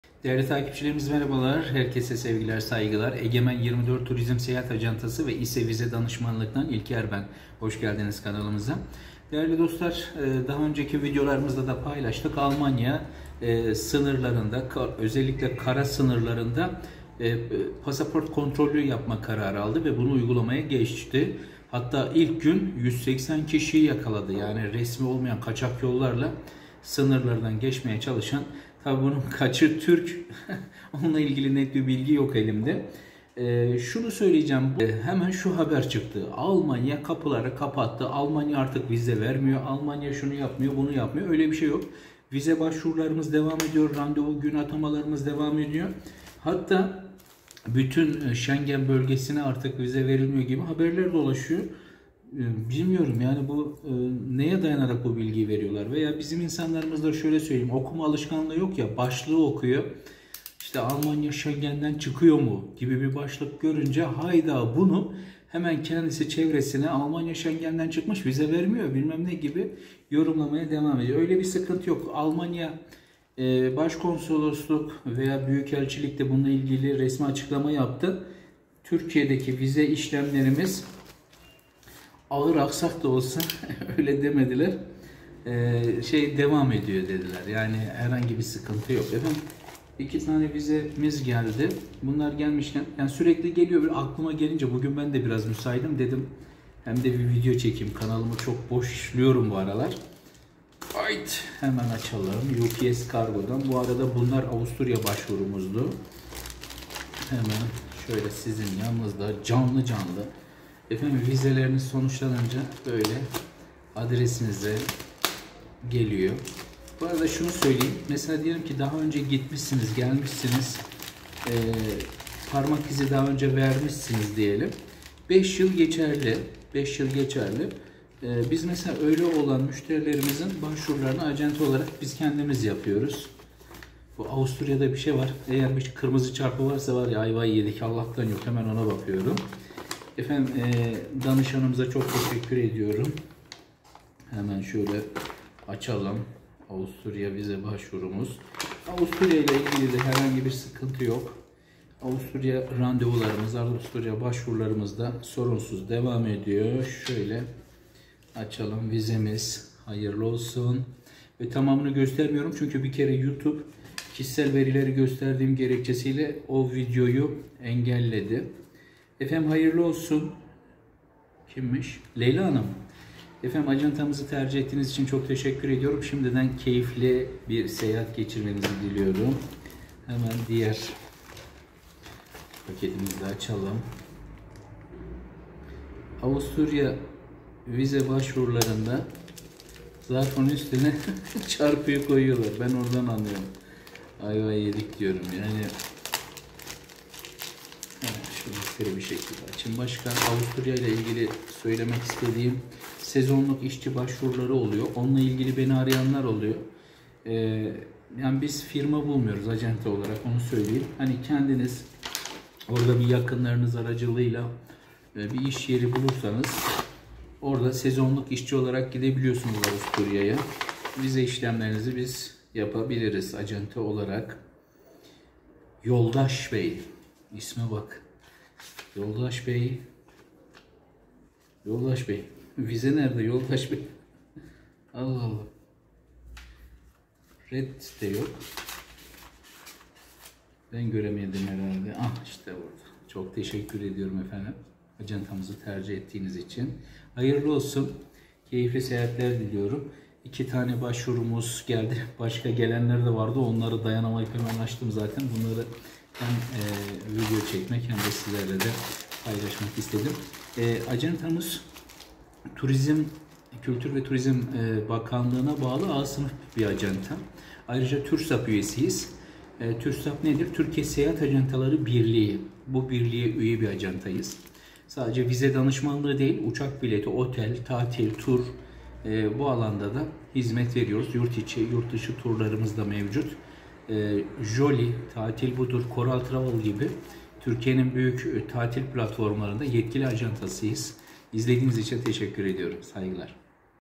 Almanya Vize Vermiyor mu? | Avusturya Vizelerimiz Geldi
Almanya'nın kara sınırlarında önlem alması Almanya Vize vermiyor söylentisi yol açtı. Avusturya için gerçekleştirdiğimiz vize başvurularımız olumlu sonuçlandı.